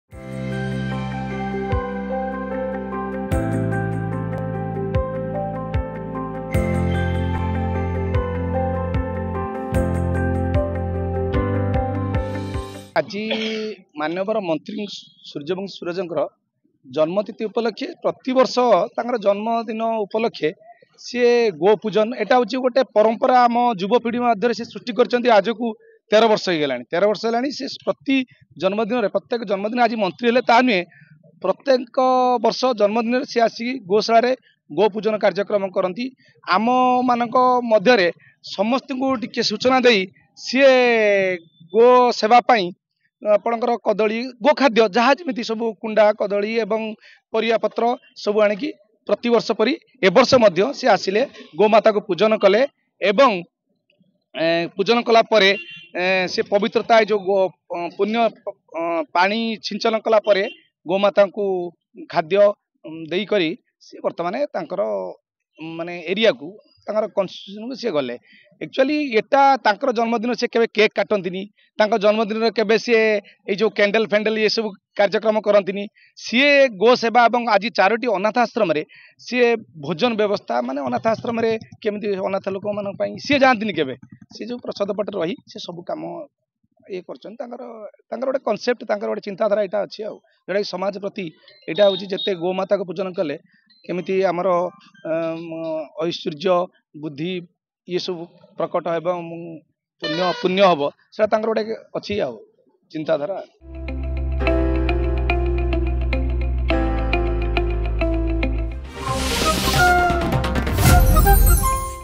Suryabanshi Suraj celebrated his birthday at Goshala | ଗୋଶାଳାରେ ଜନ୍ମଦିନ ପାଳିଲେ ସୂର୍ଯ୍ୟବଂଶୀ ସୁରଜ
Suryabanshi Suraj celebrated his birthday at Goshala
ଗୋଶାଳାରେ ଜନ୍ମଦିନ ପାଳିଲେ ଉଚ୍ଚଶିକ୍ଷା ମନ୍ତ୍ରୀ ସୂର୍ଯ୍ୟବଂଶୀ ସୁରଜ